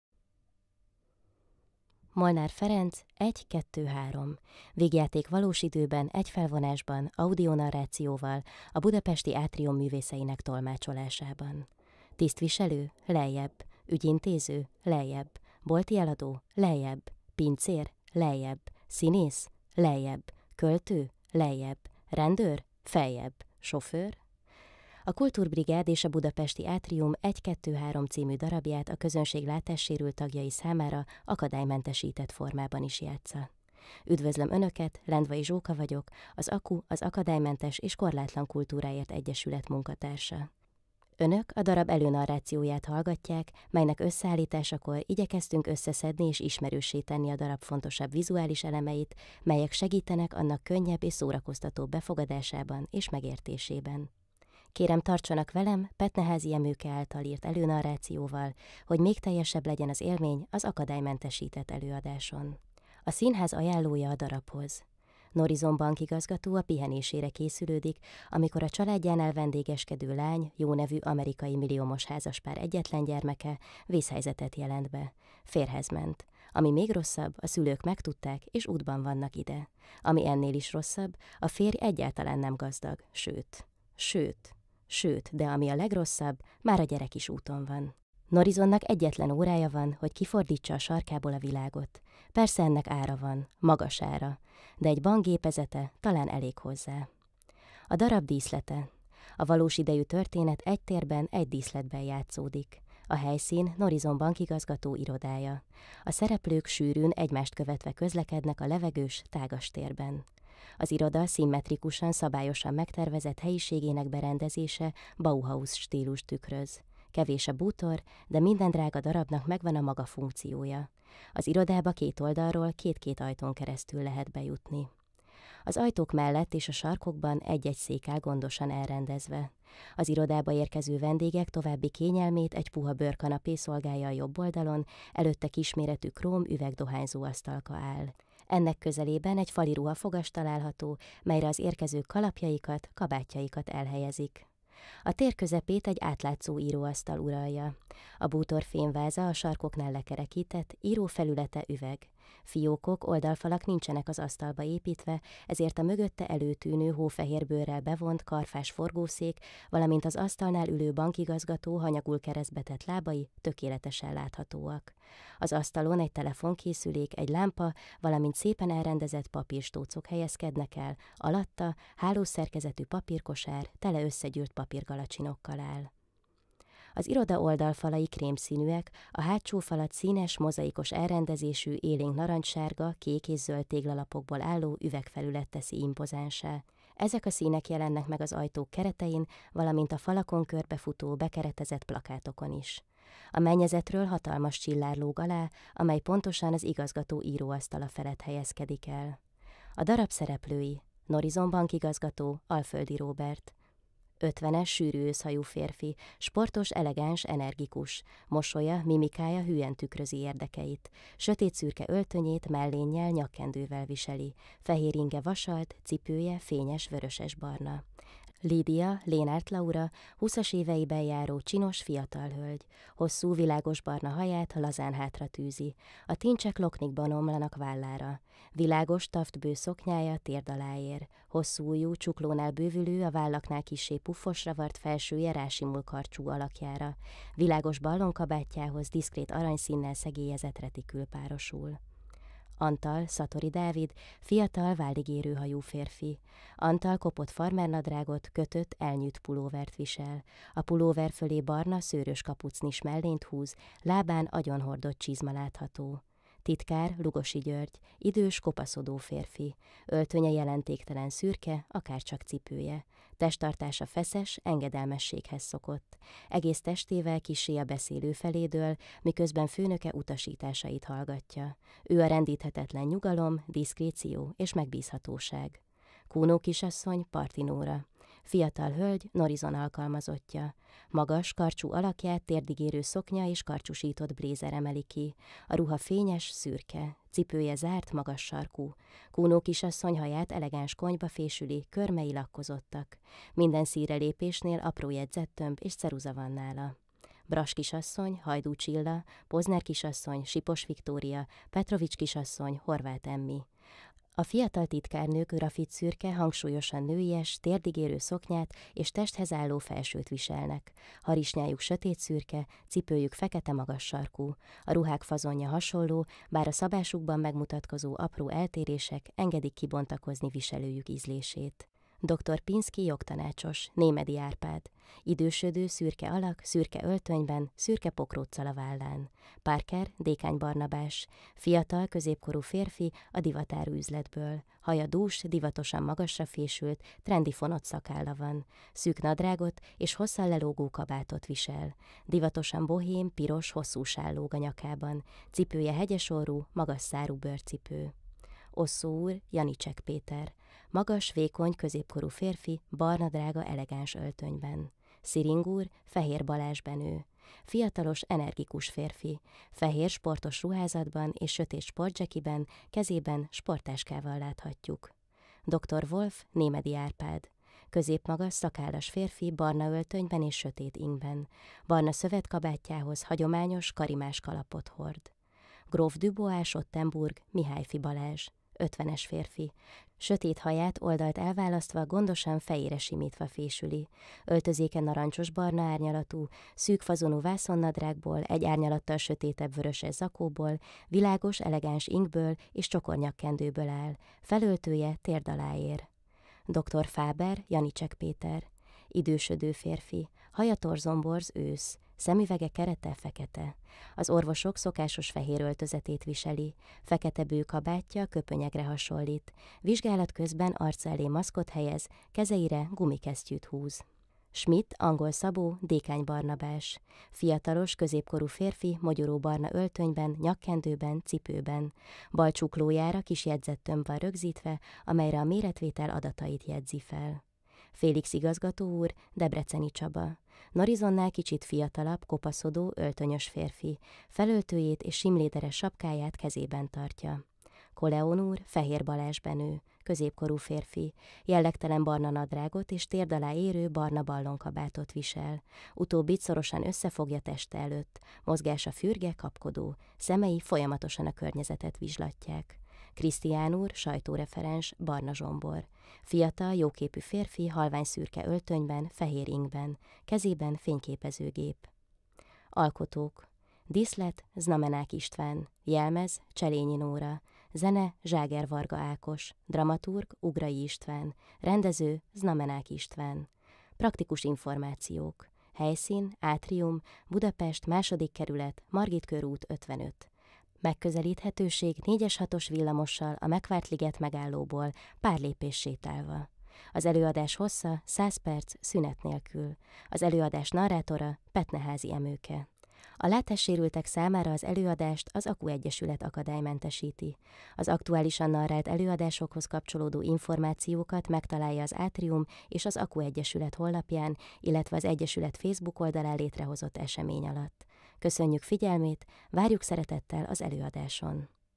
Egy, kettő, három - előnarráció
Molnár Ferenc: Egy, kettő, három
Vígjáték valós időben, egy felvonásban, audionarrációval,
a budapesti Átrium művészeinek tolmácsolásában.

Látássérülteknek az előadást akadálymentesíti az AKKU – Az Akadálymentes és Korlátlan Kultúráért Egyesület. Az aktuálisan narrált előadásokhoz kapcsolódó információkat megtalálja  az AKKU Egyesület honlapján, illetve az egyesület Facebook oldalán létrehozott esemény meghívóban.